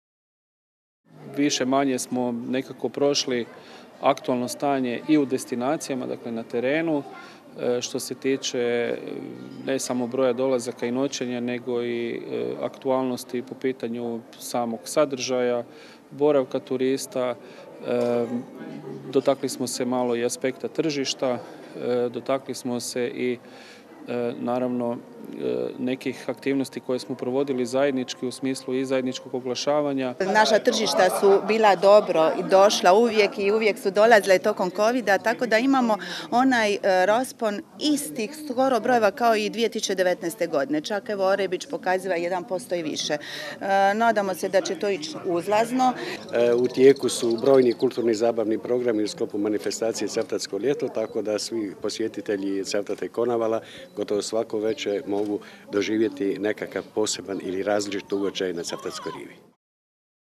Sastale se turističke zajednice DNŽ, evo što su zaključili!
A u Cavtatu je održan koordinacijski sastanak turističkih zajednica Dubrovačko-neretvanske županije. Predstavljen je dosadašnji tijek sezone, rezultati turističkog prometa kao i trendovi i očekivanja.